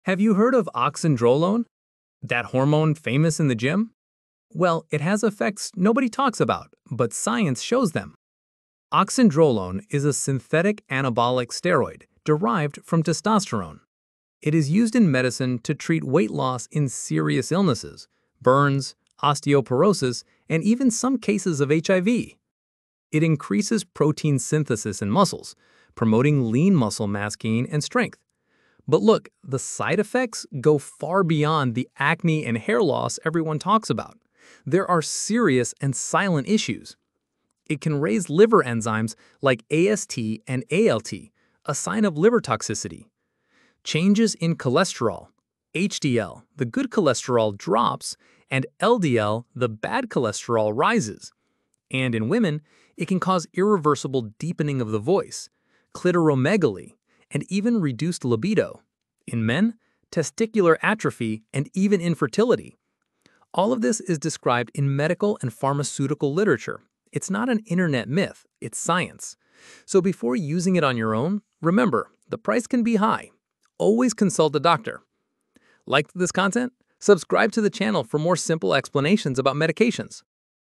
Oxandrolone Side Effects Nobody Talks About! | What Science Says
Have you heard of Oxandrolone? This popular anabolic steroid used in gyms has hidden side effects that most people don’t know about. From liver toxicity to hormone changes, science reveals the truth behind this powerful drug. Learn why it’s important to consult a doctor before using it!